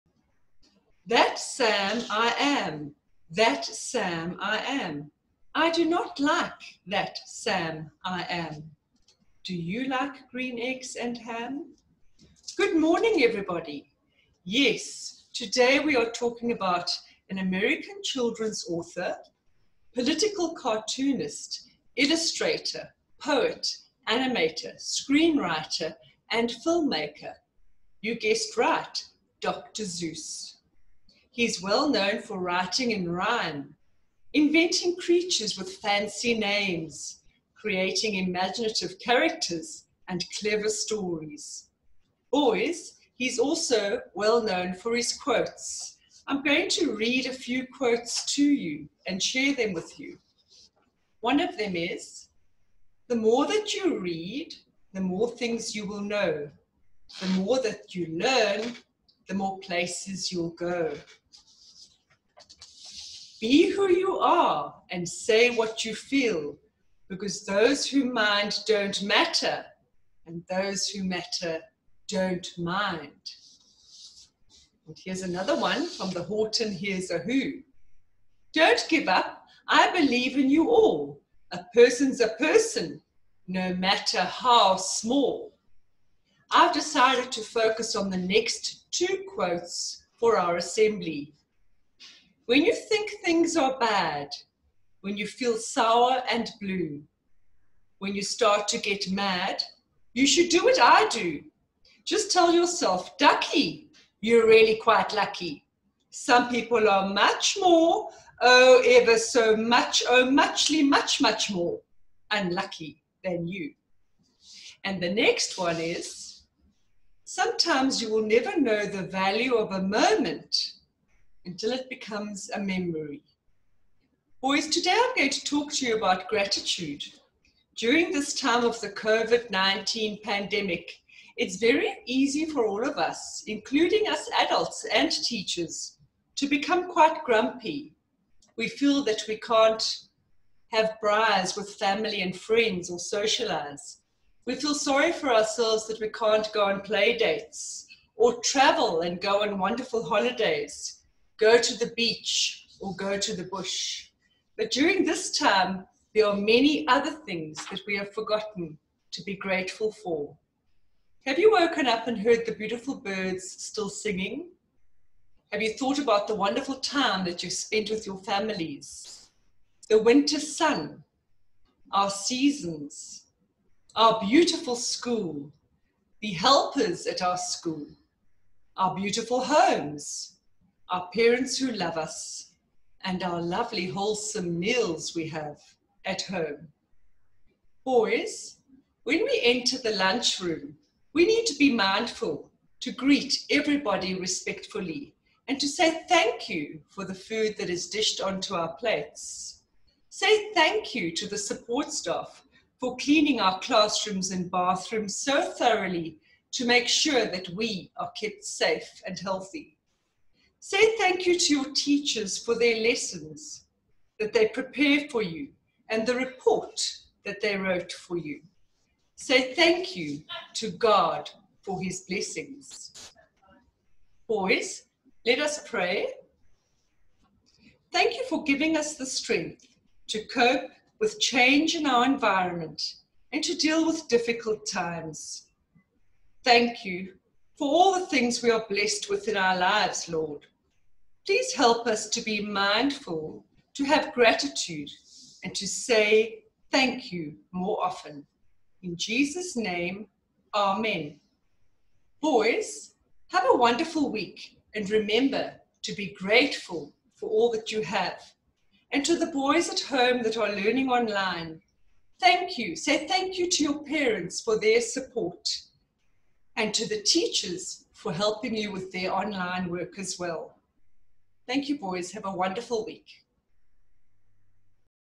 WHPS Assembly - 13 July 2020
Today's assembly is presented by our Head of Junior Prep Department  - Delaille Sass
Gratitude - "...you're really quite lucky!..." - Dr Seuss